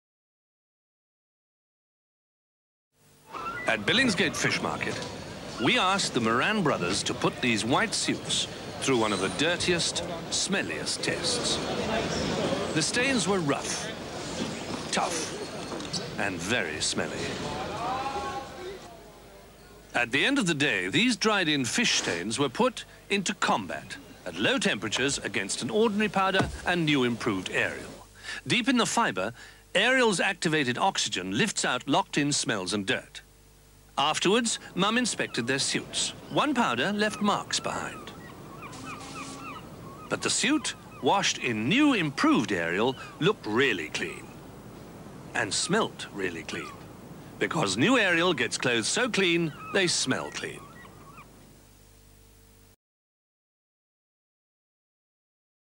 1989: Ariel Automatic [Billingsgate Fish Market]
Title: Billingsgate Fish Market
Brand: Ariel
Advertiser/Client: Procter & Gamble
Agency: -
Media: TV Advert
Featuring: -
Guest Appearance: Mrs Moran & The Moran Brothers
Voice Over: -
Year: 1989
Location: Billingsgate
Country: UK
Washing Machine: Hoover Logic
Model: -
Slogan: So Clean It Smells Clean.

(Repost: Originally uploaded on The Laundry Lab, February 2011)